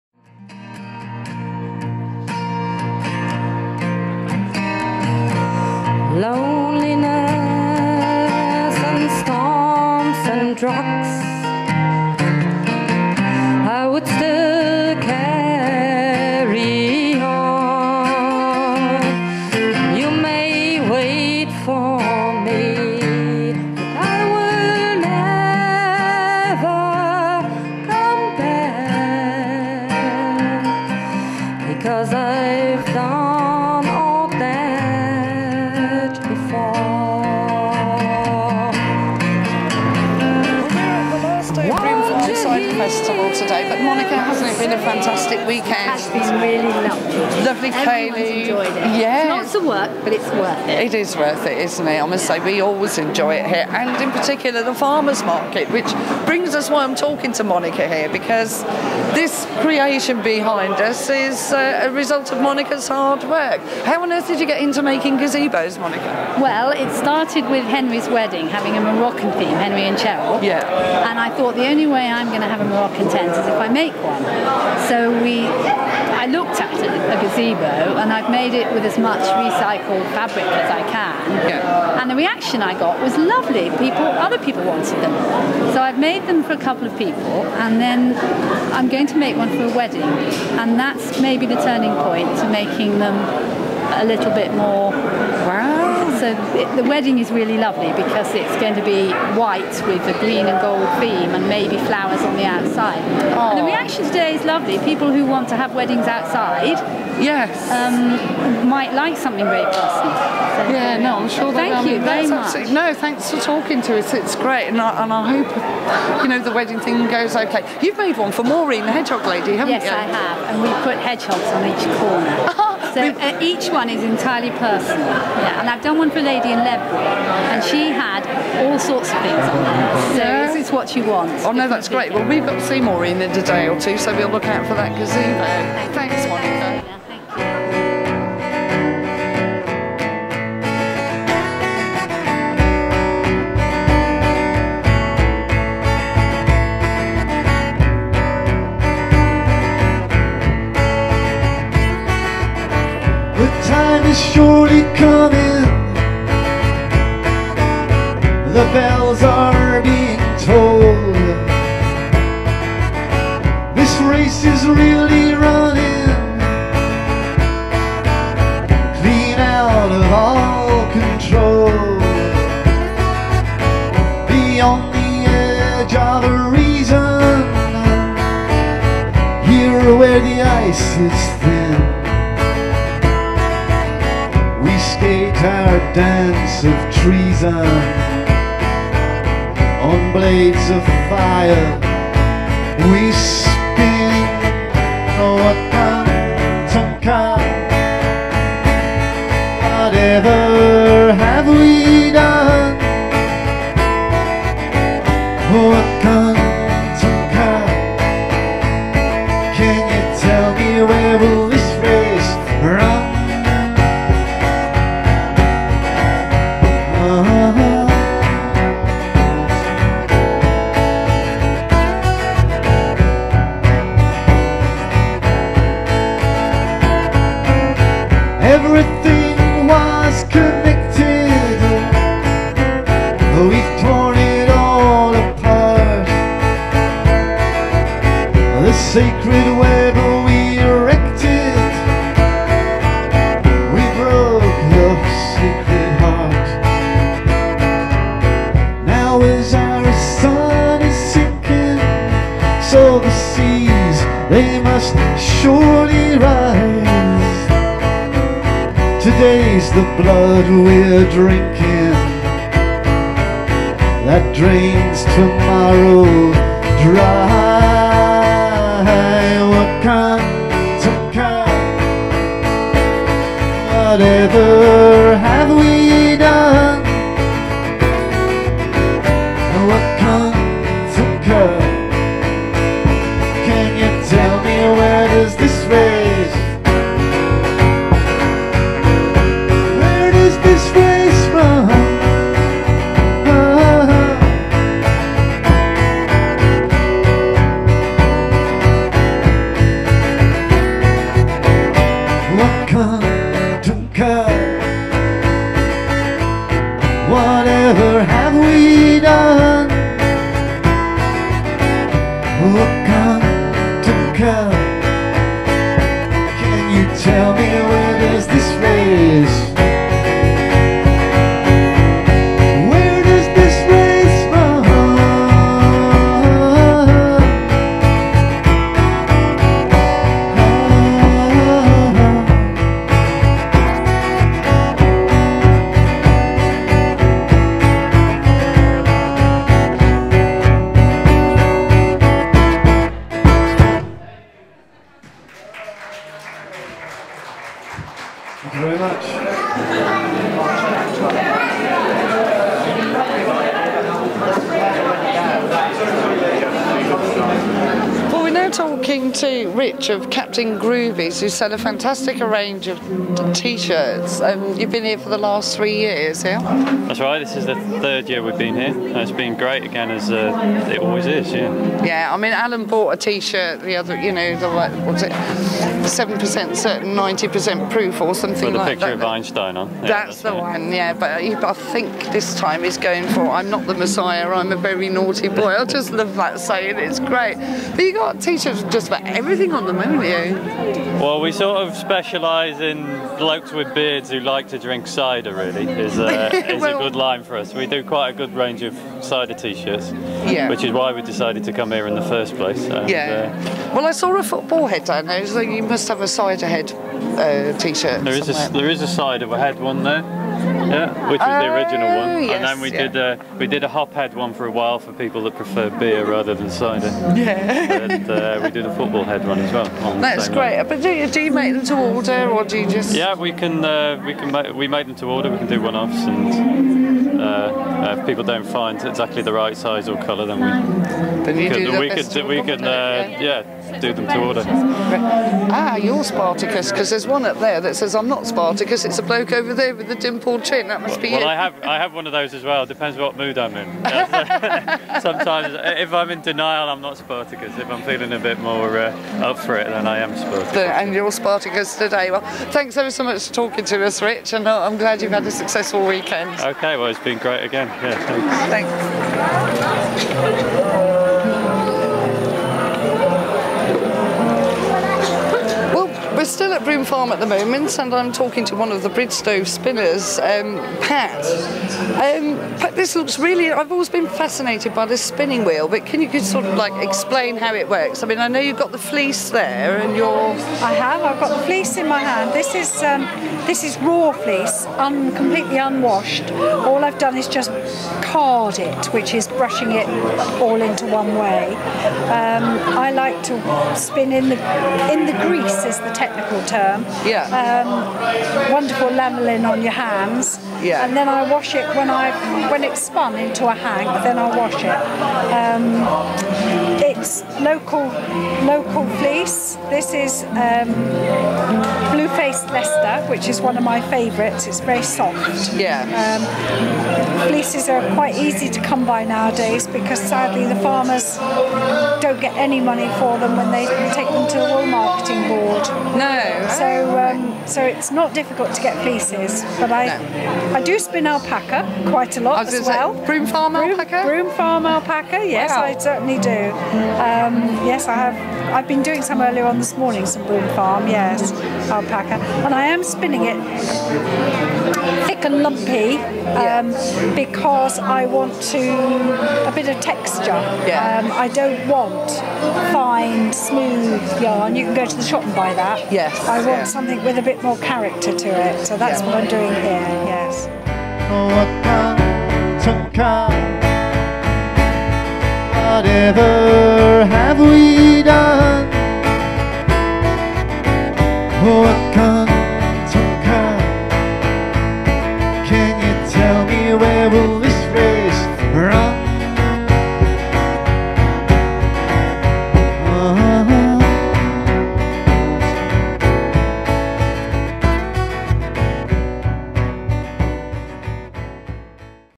Sunday Afternoon at Ross Cider Festival
Sunday at the 2009 Ross-on-Wye Cider Festival with music from Steve O'Kane and Fiona McBain. Meet Pat Brown spinning alpaca wool and others at the festival, which takes place annually at Broome Farm, Peterstow, Herefordshire on the first weekend after the August Bank Holiday.